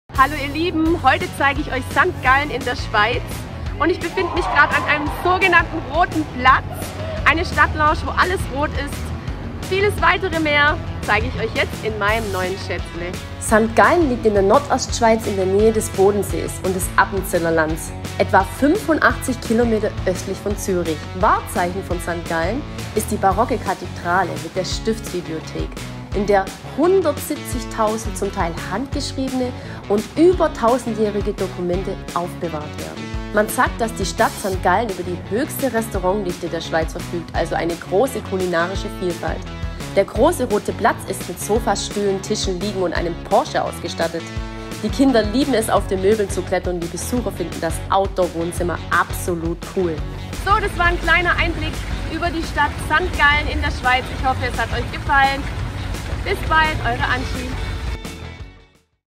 Städtetrip St.Gallen
In St. Gallen sieht Angi rot! Und noch viel mehr. Sie nimmt euch mit auf einen Städtetripp, der sich nicht nur zum Anschauen lohnt. By the way, wann wart ihr das letzte Mal in der Schweiz?